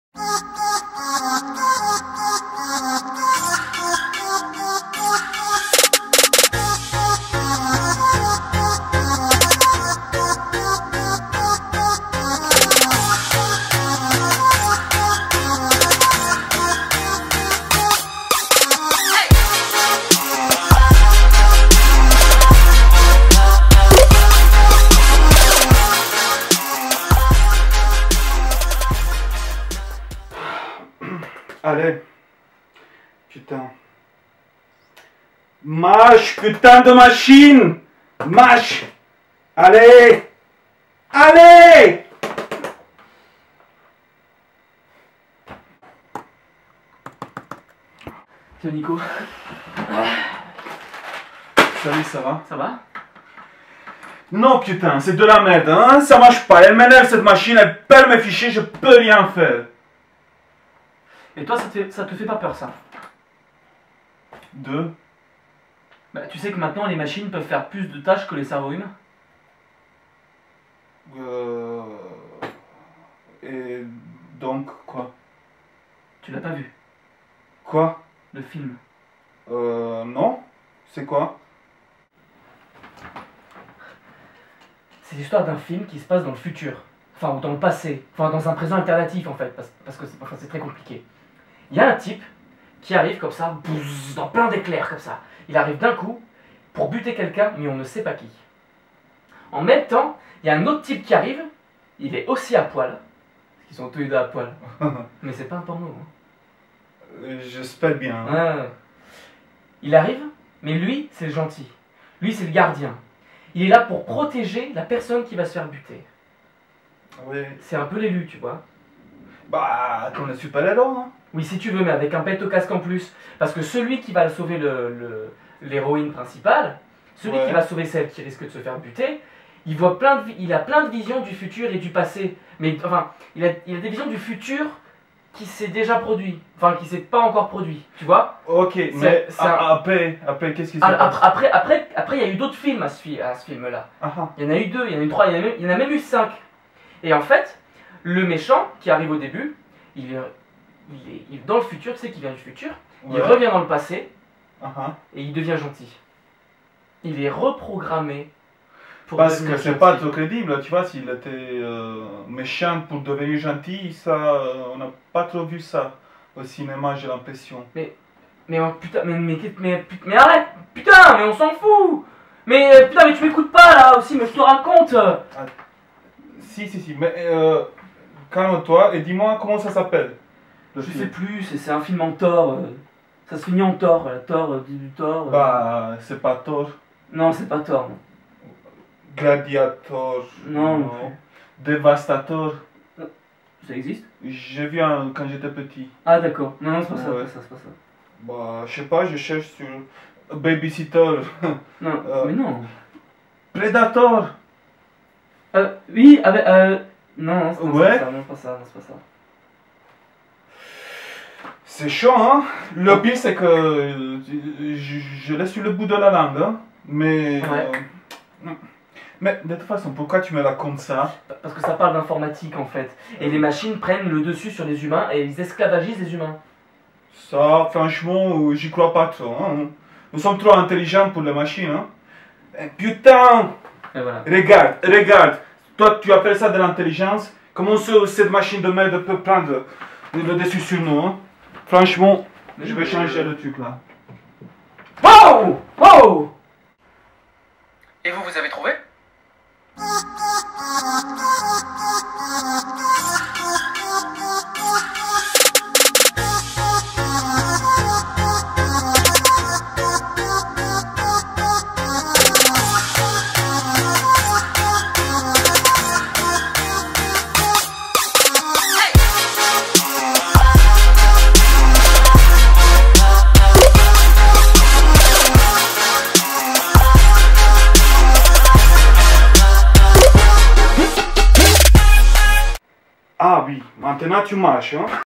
Histoire d'un Film- Geek en colère (épisode 3)
Nico a du mal avec son ordinateur, son ami en profite pou lui parler d'un Film...